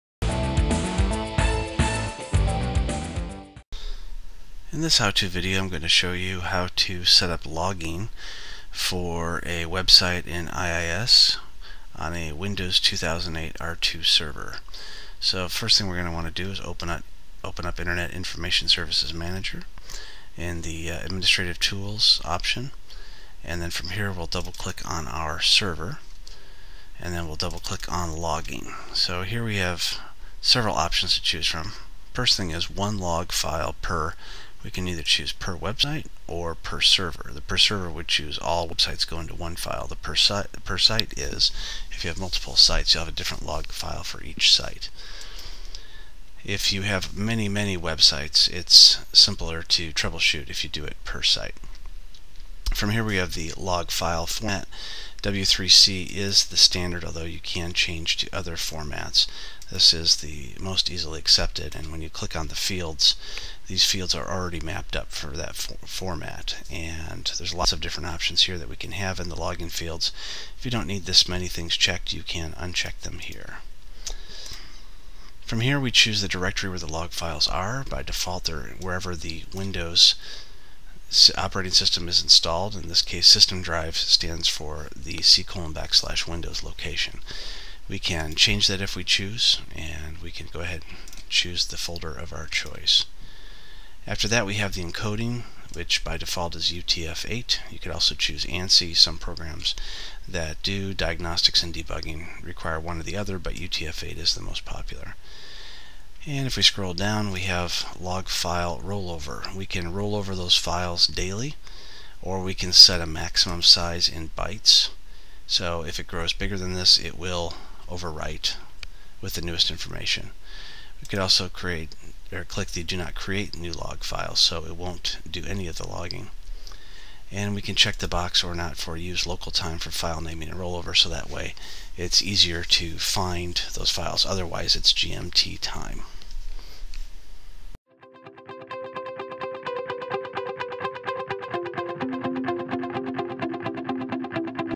How to setup and edit logging in IIS on a Windows 2008 R2 server
Author and talk show host Robert McMillen explains how to How to setup and edit logging in IIS on a Windows 2008 R2 server.